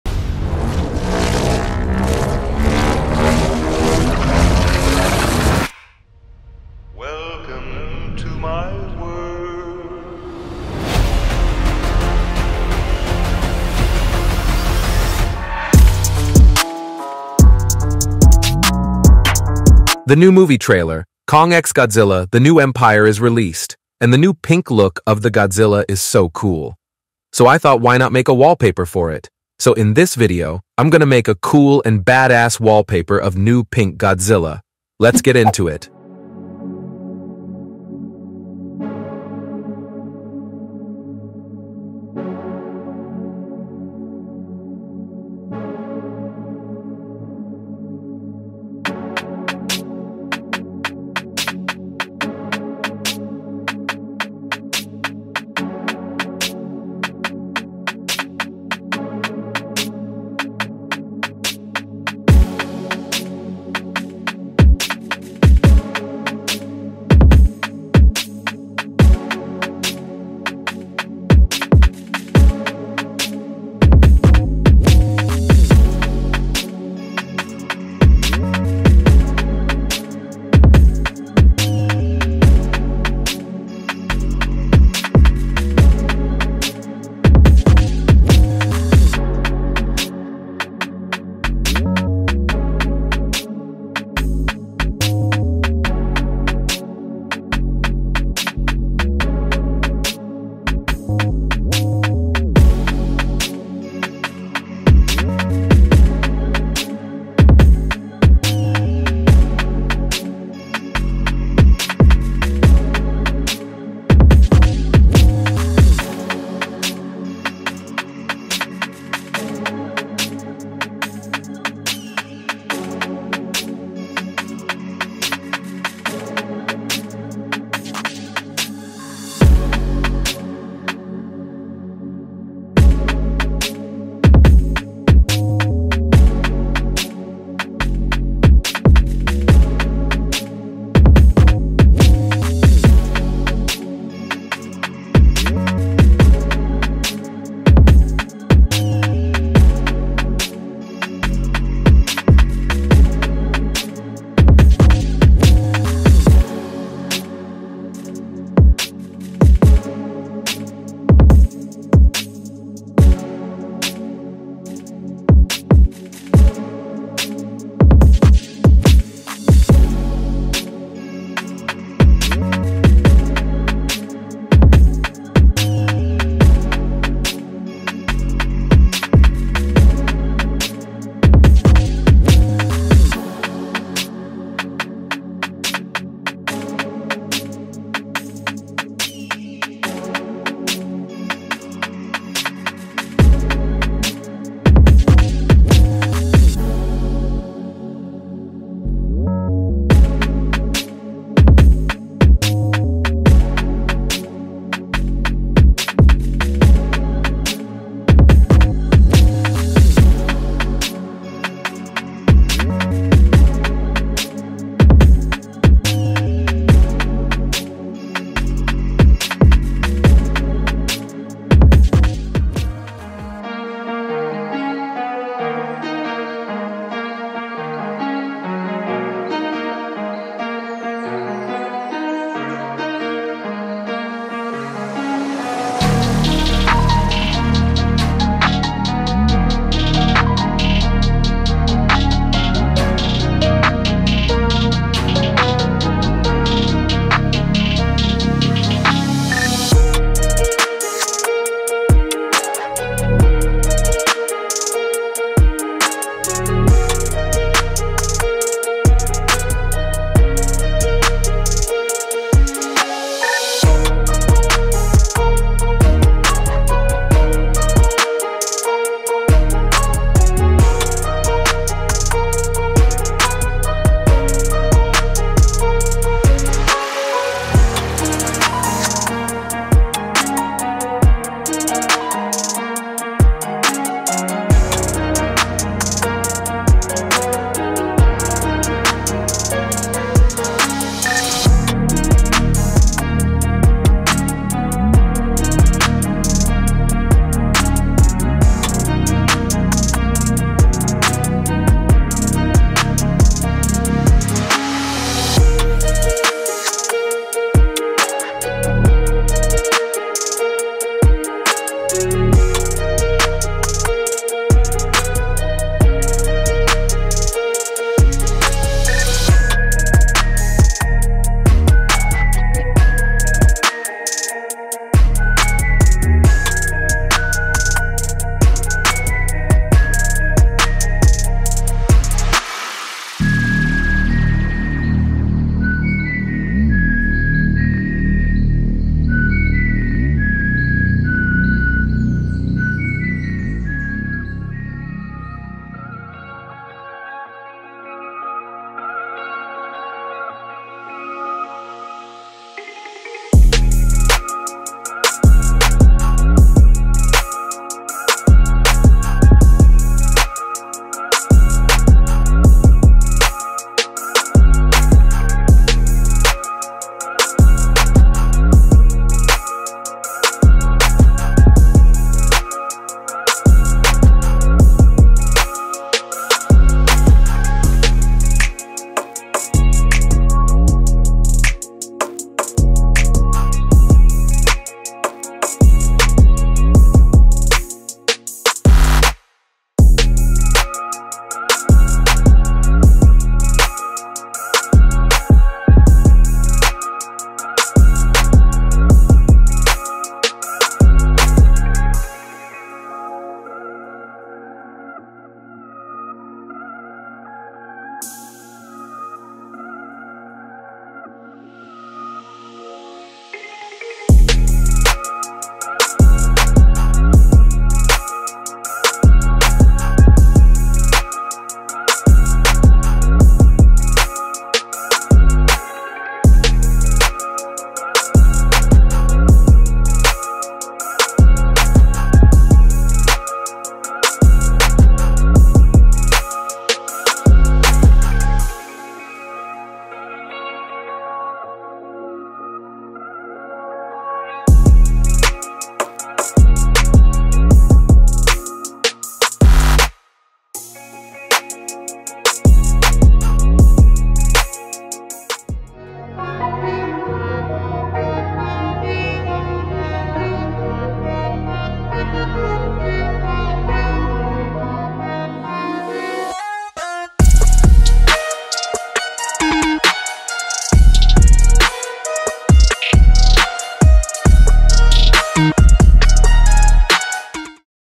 How I made an Epic Artwork for Godzilla x Kong the New Empire | Using Photoshop
How to make pink godzilla Artwork in Photoshop. Pink Godzilla Photoshop Artwork. Godzilla x Kong the New Empire Photoshop Artwork.

In This Video:
I'm gonna make a Pink Godzilla Wallpaper in Photoshop. Watch the video till the end you'll definitely gonna learn something new.

Together, let's push the boundaries of creativity and explore new realms of artistic expression.

Timestamps:
Intro: 0:00
Editing: 0:35
Outro: 7:47